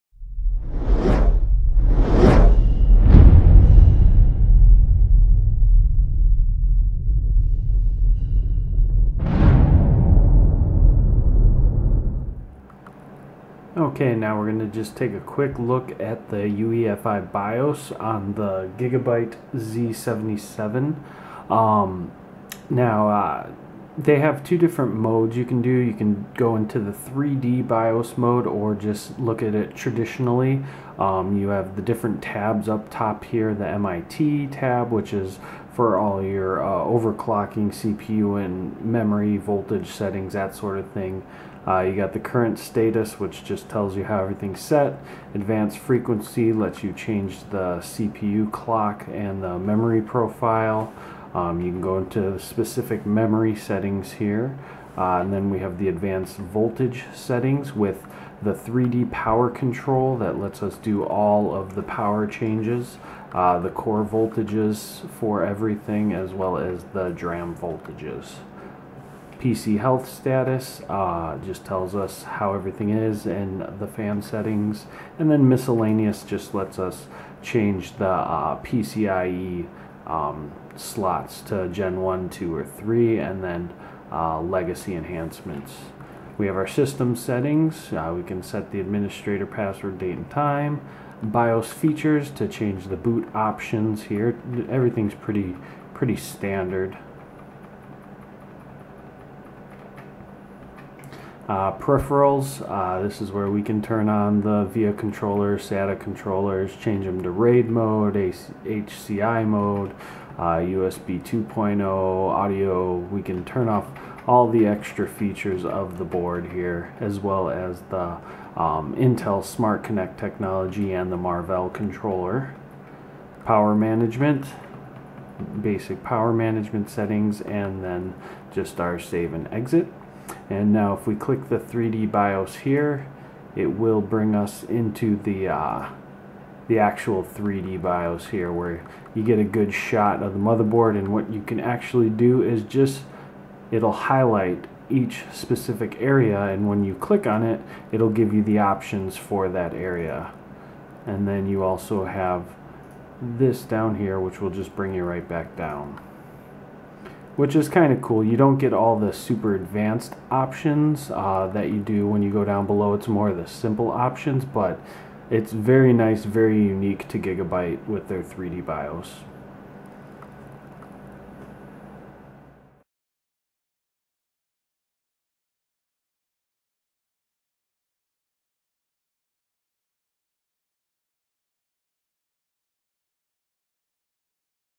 Gigabyte Z77X-UD5H-WB WiFi 3D Dual UEFI BIOS Overview + Tutorial
Great offer on the Gigabye Z77-UD5H-WB WiFi

Skataneric gives an overview of the 3D Dual UEFI BIOS on the Gigabyte Z77X-UD5H.

The Gigabyte Z77X-UD5H-WB is a uniquely designed motherboard that straddles the line between mainstream and high-end. The Gigabyte Z77X-UD5H-WB is priced at $209, placing it within the mainstream segment but it is loaded with features that are only often found in high-end motherboards. Capable of PCI-E 3.0 multi-GPU setup up to TRI-SLI/TRI-Crossfire configuration at 8x/4x/4x, the Gigabyte Z77X-UD5H-WB is a solid gaming platform as well. For users who want to squeeze out as much performance possible from their Ivy Bridge or Sandy Bridge processors, Gigabyte has made it convenient with the addition of on-board buttons, voltage readout points, debug LED, dual-BIOS and an on-board BIOS switcher on the Z77X-UD5H PCB. Optional extra power can be added to the PCI-E as well through the OC-PEG connector, useful for extreme LN2 GPU overclocking. In terms of USB connectivity, the Gigabyte Z77X-UD5H has three on-board USB 3.0 headers (2x VIA VL810, 1x Intel Z77) for up to six ports as well as four more USB 3.0 ports in the back. There are also two Gigabit LAN ports which use an Intel WG82579V, as well as an Atheros AR8151 controller. For wireless networking, Gigabyte has also bundled a Bluetooth 4.0/Dual-band WiFi card, capable of fast transfer rates up to 300 Mbps.